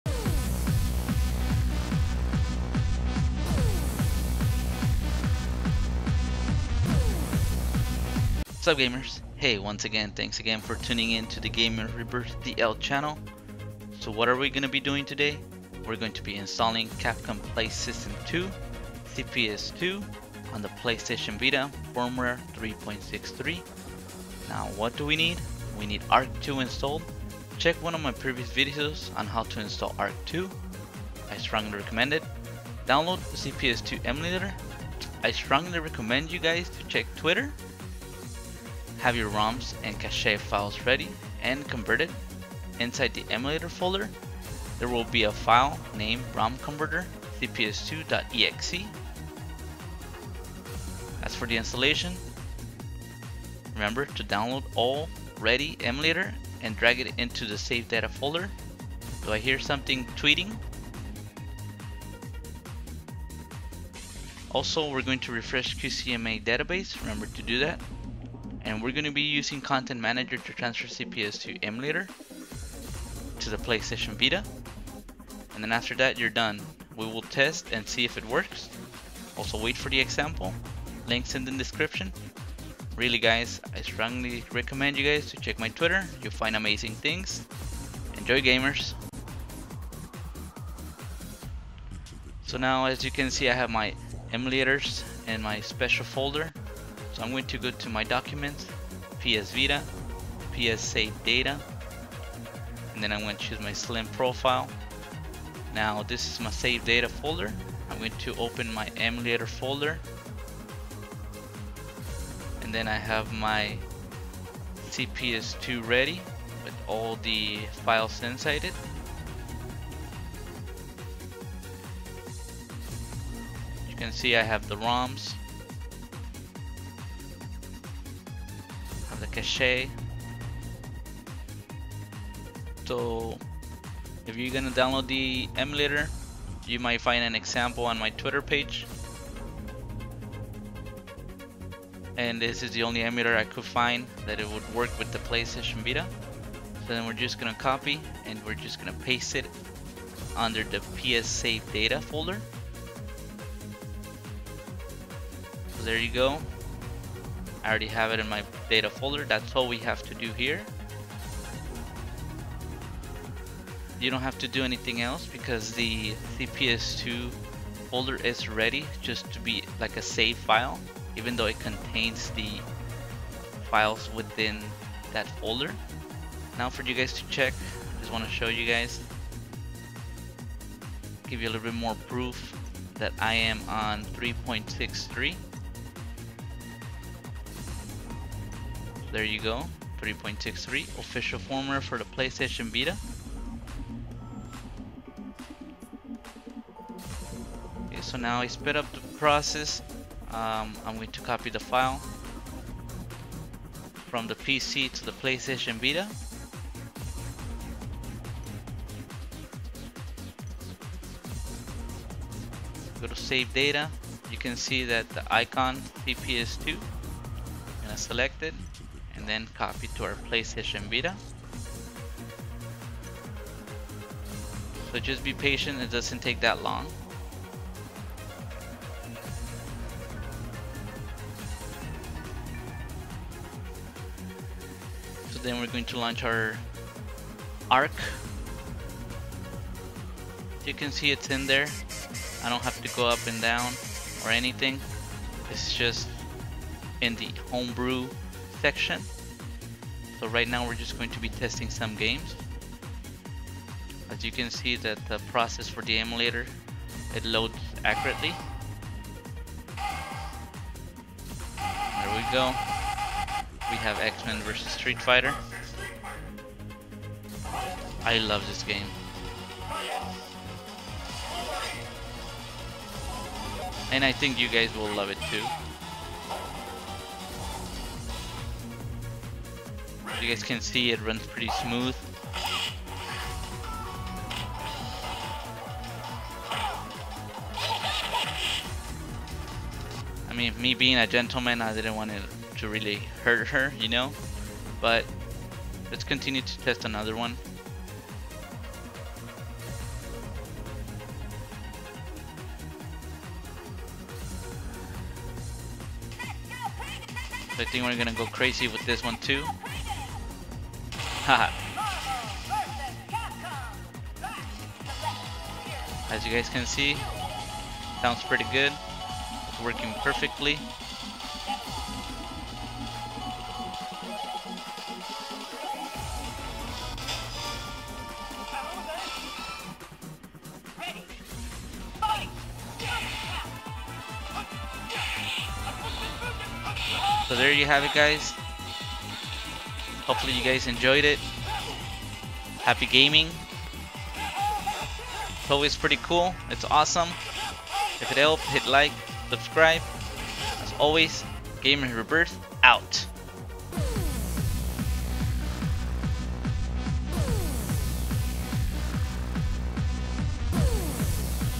INSTALLING CAPCOM PLAY SYSTEM 2 (CPS2) ON PS VITA FW 3.63! EASY INSTALLATION!
*INSTALLING CAPCOM PLAY SYSTEM 2
(CPS2) ON PS VITA FW 3.63!*
What you need:
ARK-2 Installed (Check my previous video on how to install ARK-2)
Download CPS2 Emulator ;) (I Strongly Recommend to Check Twitter)
Have your Roms & Cache files ready and converted… Inside the Emulator folder it will have the file romcnv_cps2.exe
Installation:
Download the All Ready Emulator and drag it into the SAVEDATA folder “tweet”
Refresh QCMA’s Database
Use Content Manager to transfer CPS2 Emulator File to PS Vita
You are DONE! We will test and see if it works!!! Wait for Example
P.S. Remember to Check my twitter!
Enjoy Gamers!!!